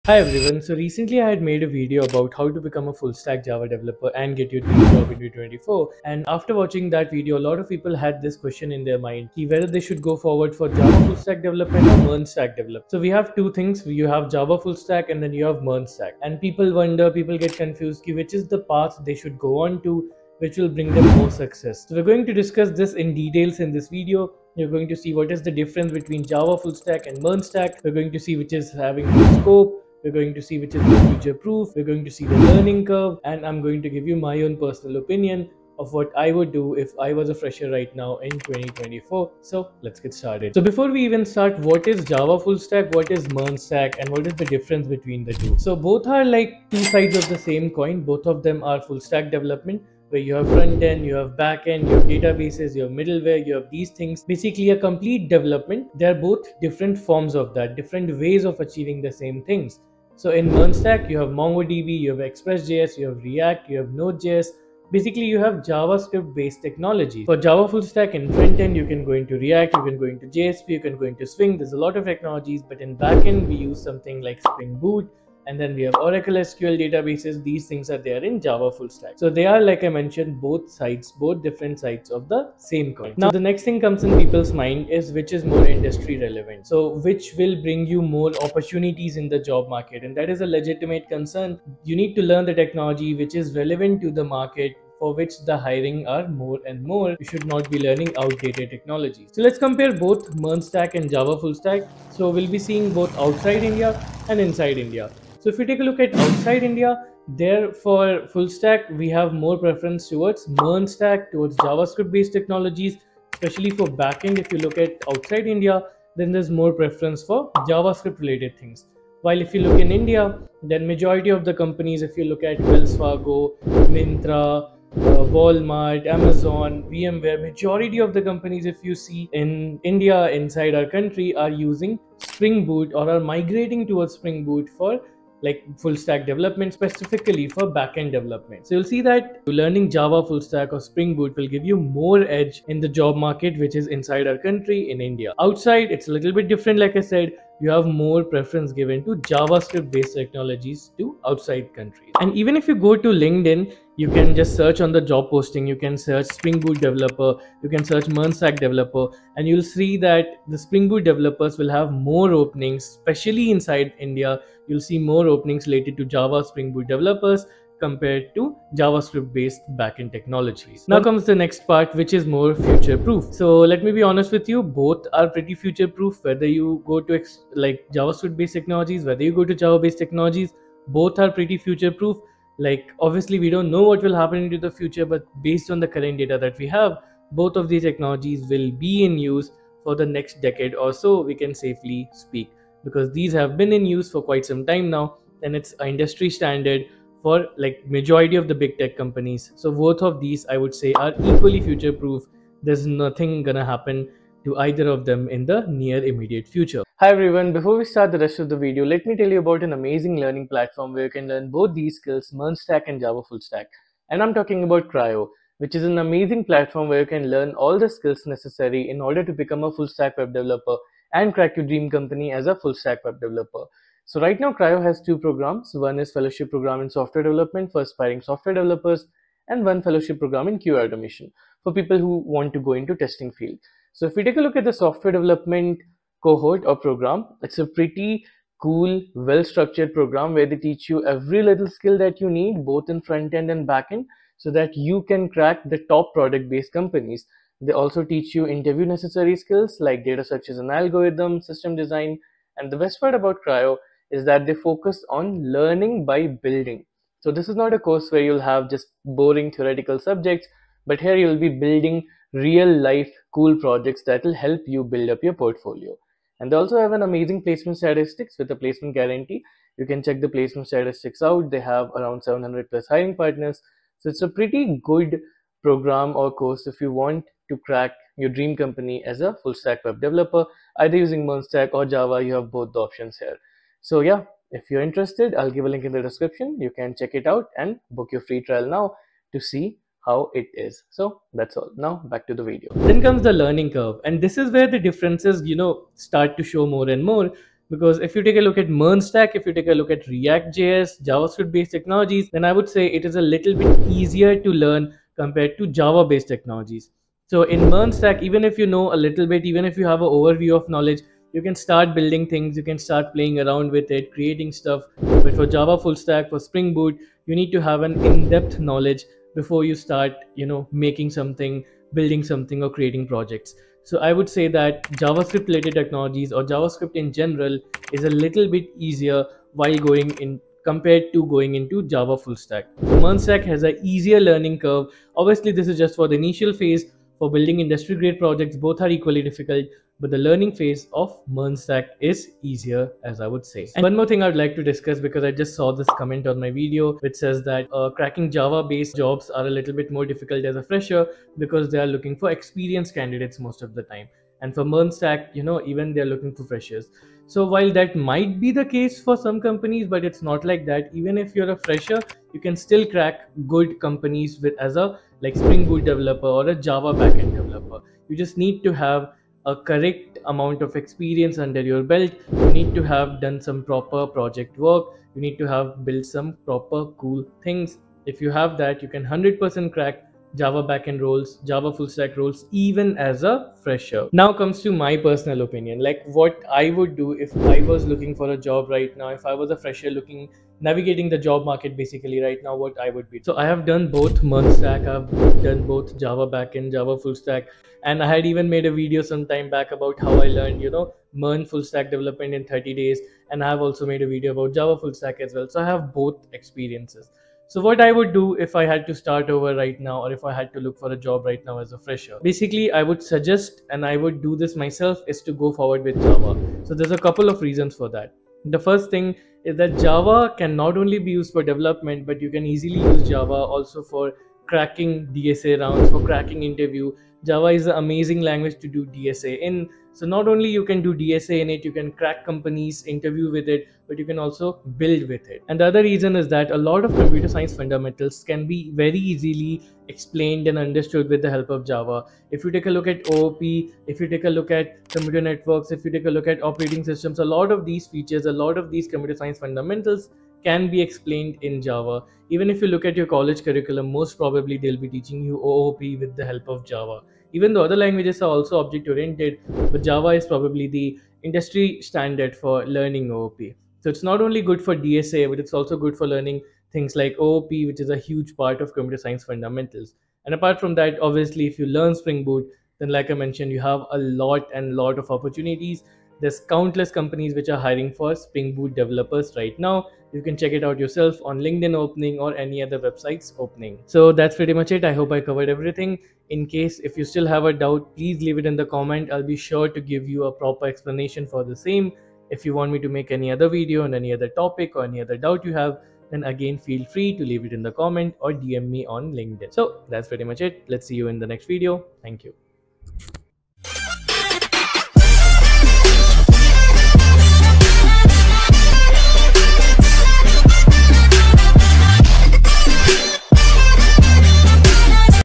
MERN vs Java Full Stack Development 2024 | Which to do?
Hi Everyone,
In this video  I have explained over the differences between MERN Stack and Java Full Stack and which one you should take in 2024.
I have discussed opportunities,future scope, learning curve etc so you can have a clear idea about which is the path you should go on.

00:00 Introduction
00:41 What is MERN/Java Full Stack
01:23 Which is having more scope?
02:50 Futureproof
03:26 CRIO
04:53 Learning Curve
05:43 Fresher Friendly
06:22 My Opinion/ Conclusions